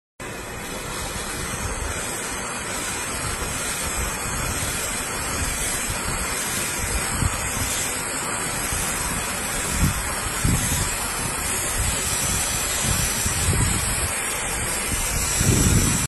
Dry vapor steam cleaning removing pollen and mildew from veranda
Order our products at htps://cleanersolutionsintl.com. Prices starting at $850.00.